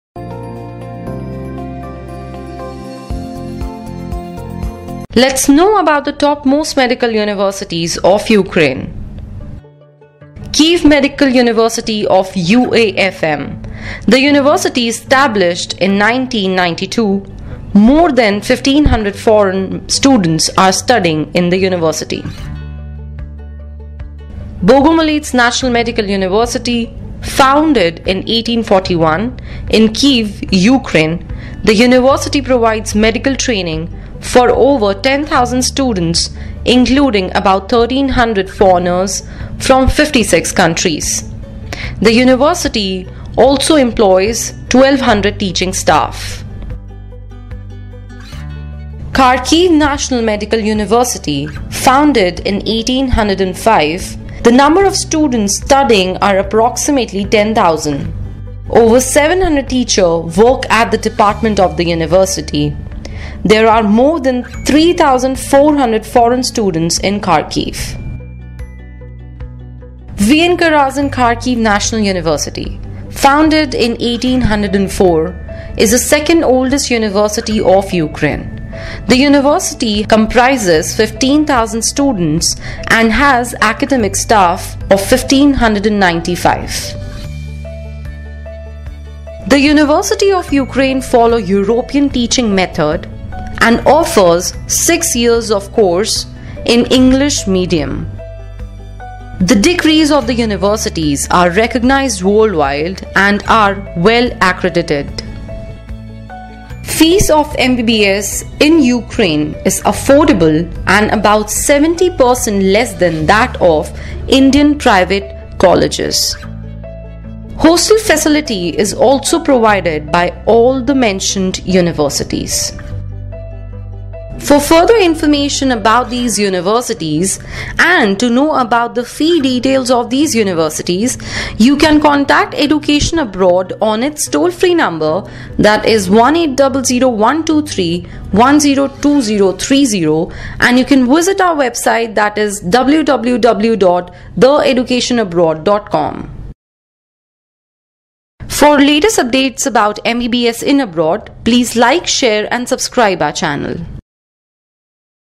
MBBS IN UKRAINE | TOP MBBS UNIVERSITIES OF UKRAINE
Many students who seek admission in MBBS in Ukraine fail to choose the right university for themselves because of the lack of knowledge and wrong guidance. This video explains which university is better for Indian Students.

Watch this Complete video to know more.